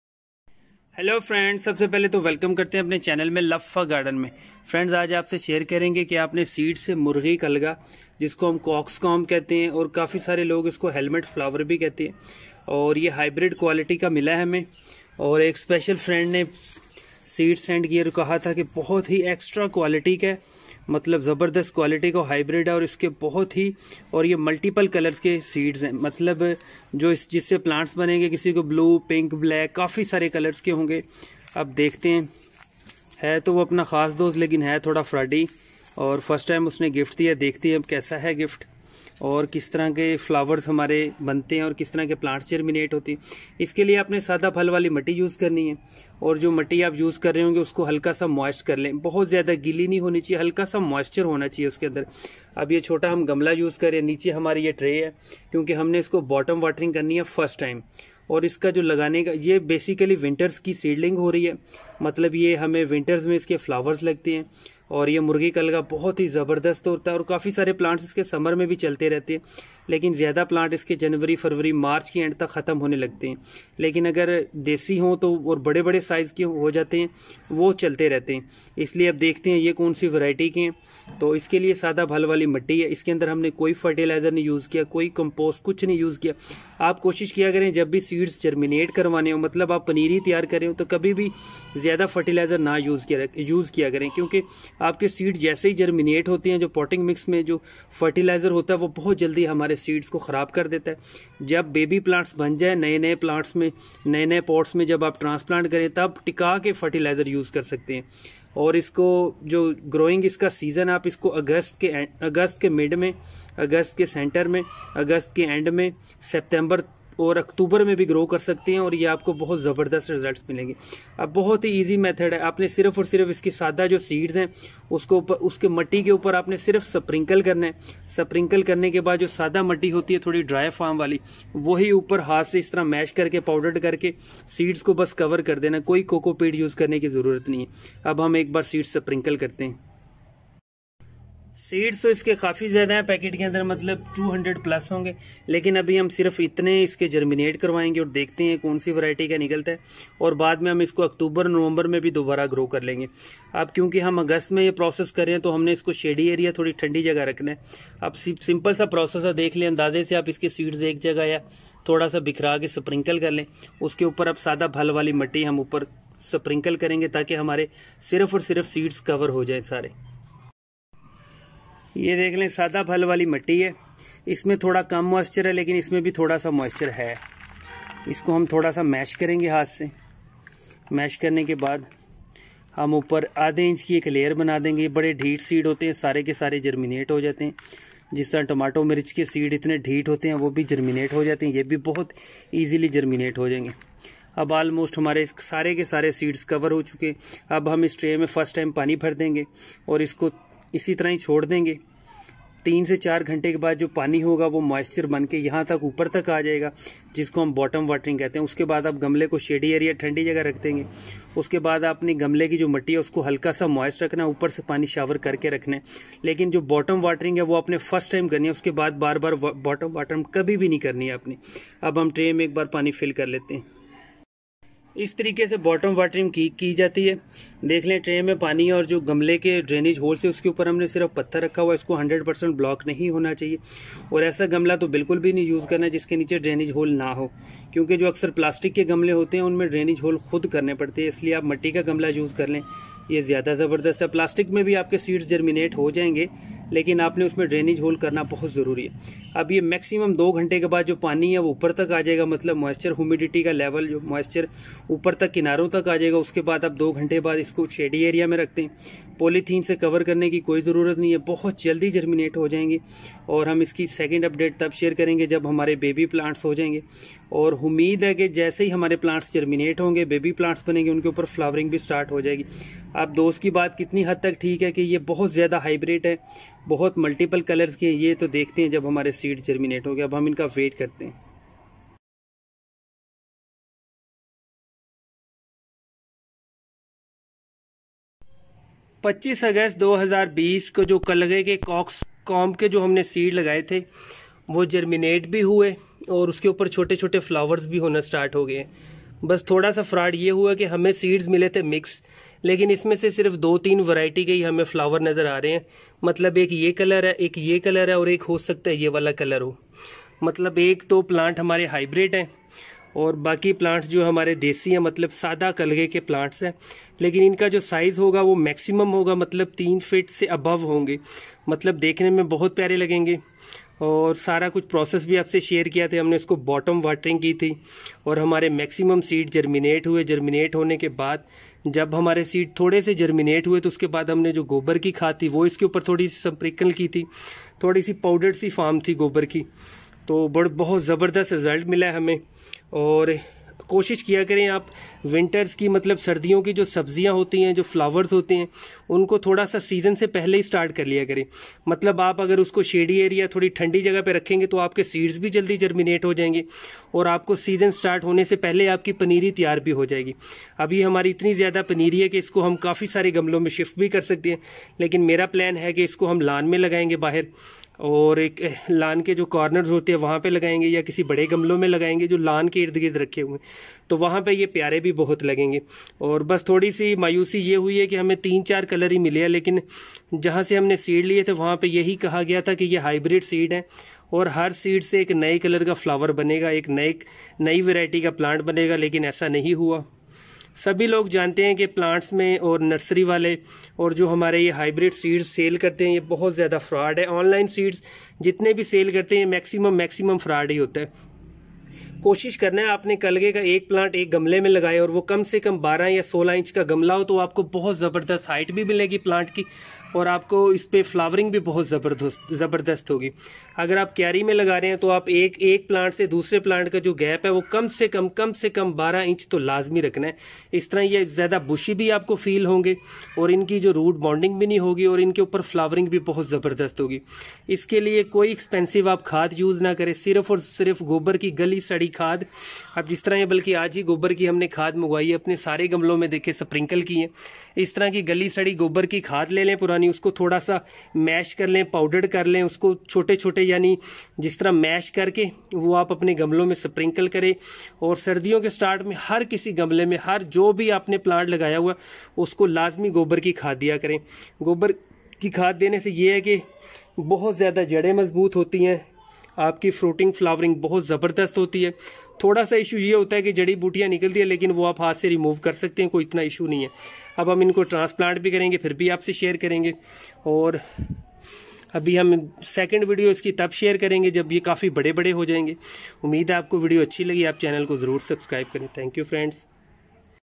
How To Grow Cockscomb Flower From Seeds. Grow Murghi kalgha Flowers. Hindi / urdu
Easy to grow from seeds
It's called helmet flowers

Grow Cockscomb flowering plants